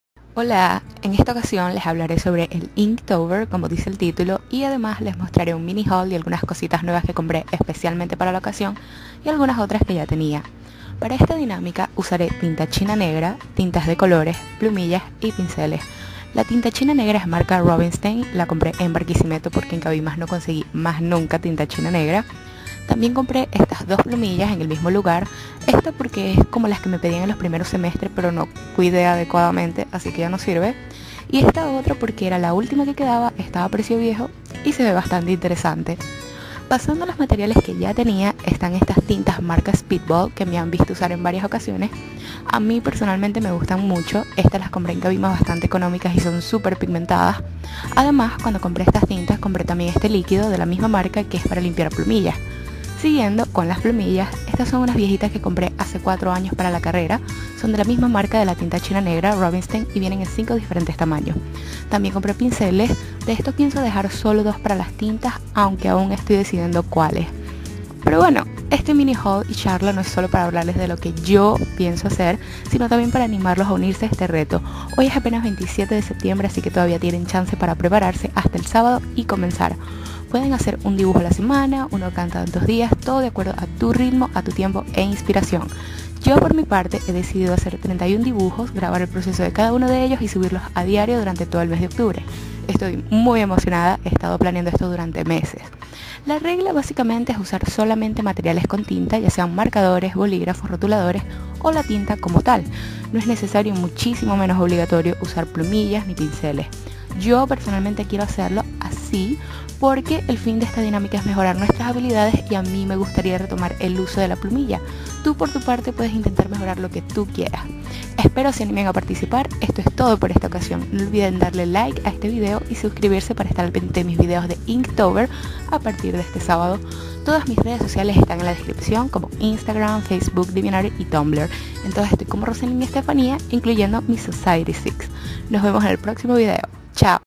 MINI HAUL • ¡MATERIALES DE ARTE! | 31 Días de INKTOBER #00
¡Hola! Este es un pequeño HAUL muy especial de materiales nuevos que compré (y algunos viejos también) para el INKTOBER de este año. ¡Espero se unan ustedes también! ♡

MATERIALES:

•Tinta Speedball de colores
•Tinta china Rubinstein negra.
•Pinceles genéricos.
•Plumillas genéricas.